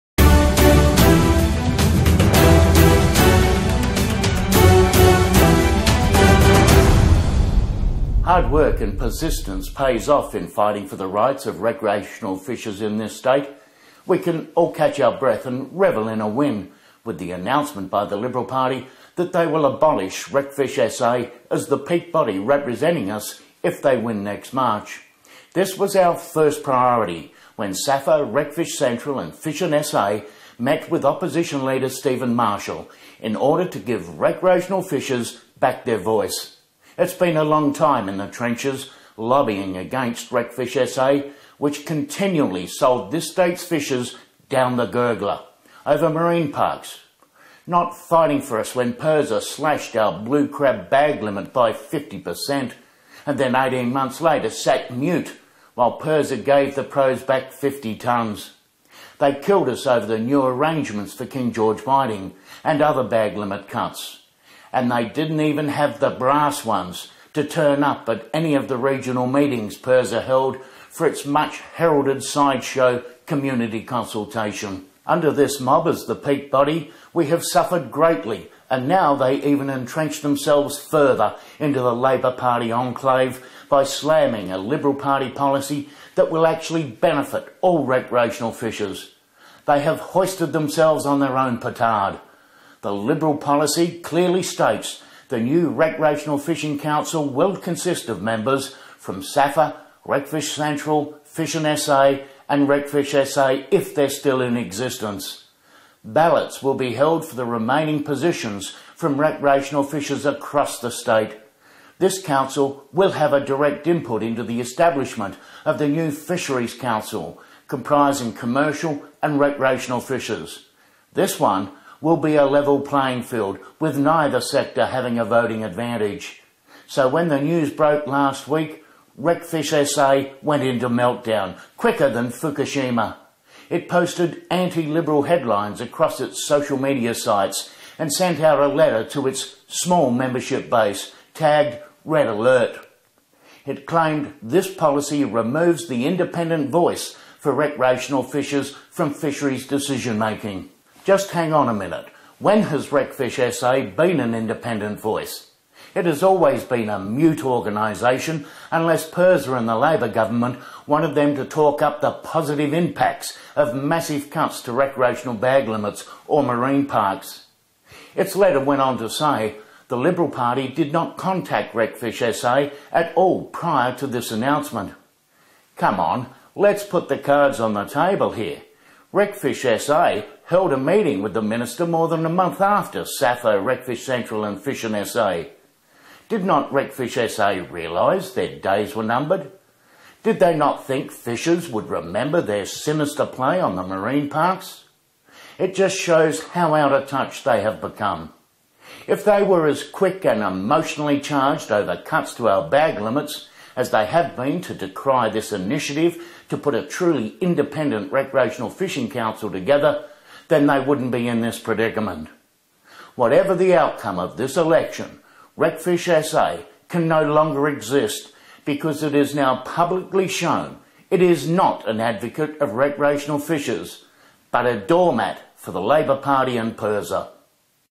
Jay Weatherill awaiting a policy
Jay Weatherill and RecfishSA is in denial over the future path of recreational fishing representation in this state.